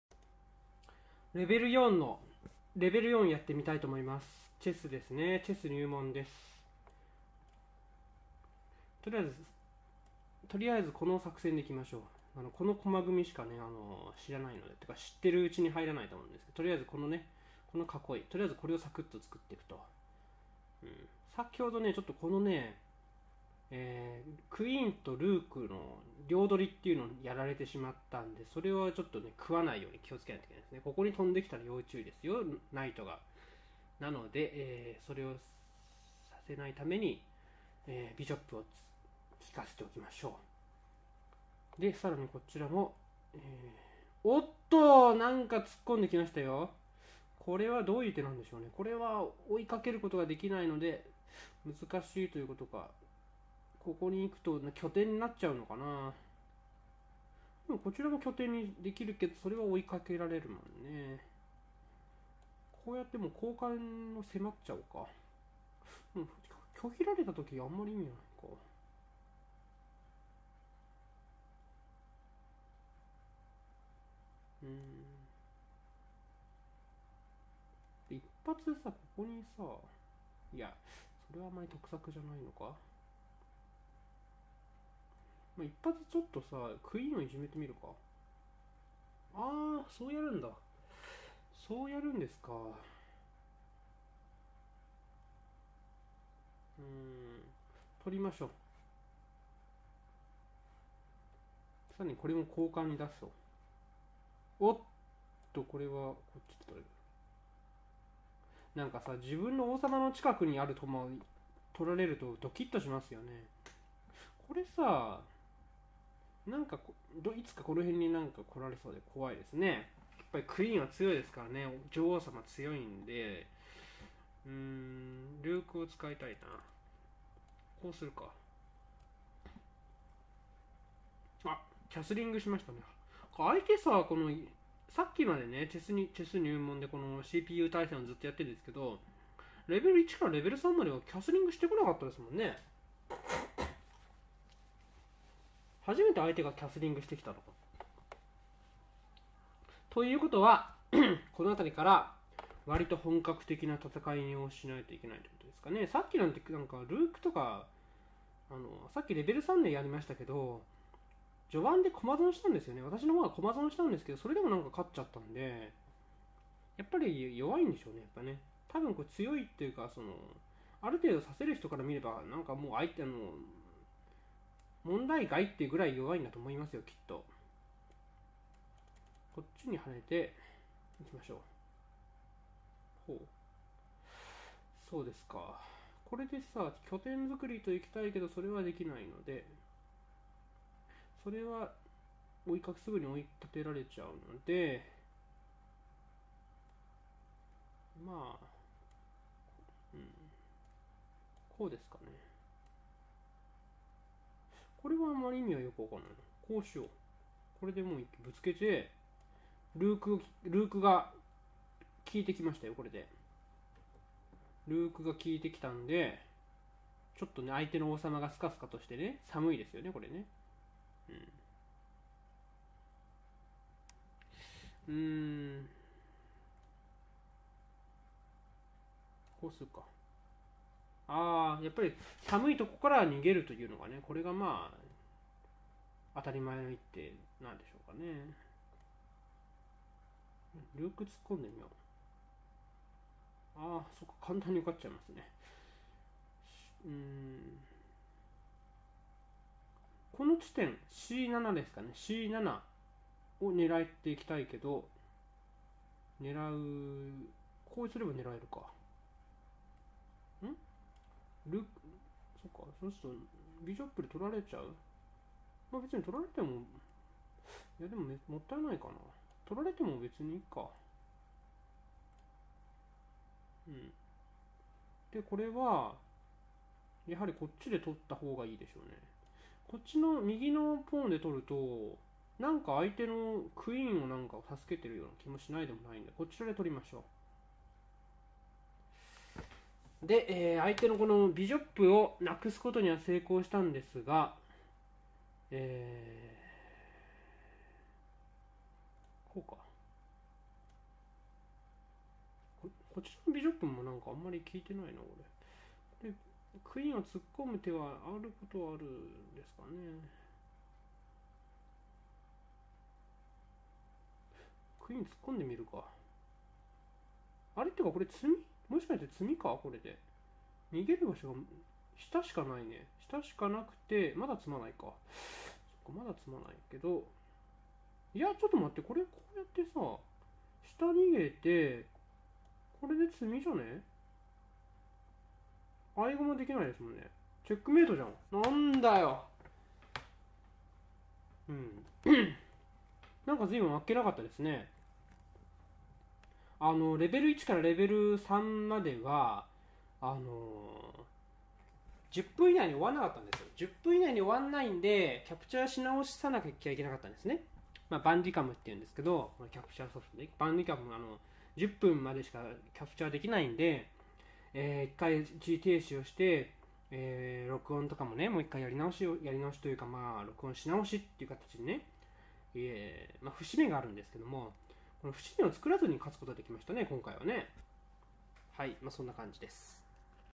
チェスに入門（レベル４）
チェス入門というサイトのCPU対戦です。チェスには持ち駒がないので、連続的に王手（チェック）できるような展開なら、すでに勝勢なのかもしれません。日本将棋のように、持ち駒が使えるバージョンのチェス、なんてのもやってみたら面白い気がするのですが、どうでしょう？